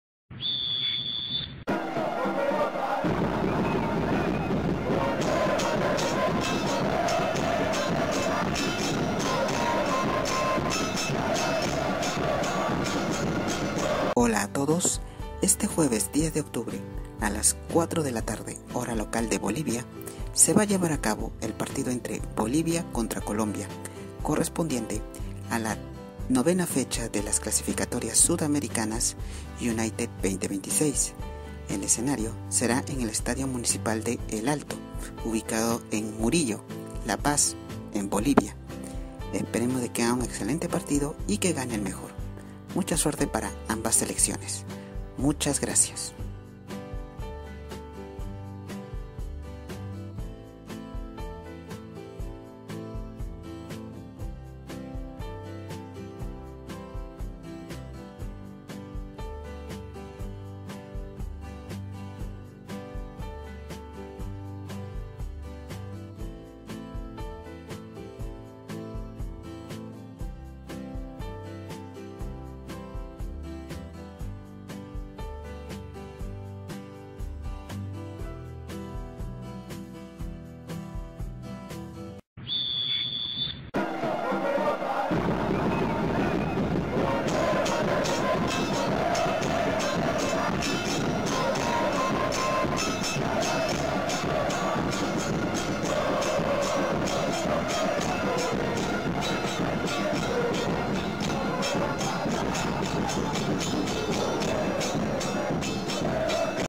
Clasificatorias Sudamericanas - Mundial UNITED 2026 (Fecha 09): Bolivia vs Colombia
Saludos amantes del deporte rey, esperemos que todos estén alentando el partido entre "Bolivia vs Colombia", correspondiente a la novena fecha de las Clasificatorias Sudamericanas rumbo al Mundial "UNITED 2026". Mucha suerte para ambas selecciones y que gane el mejor. Muchas gracias.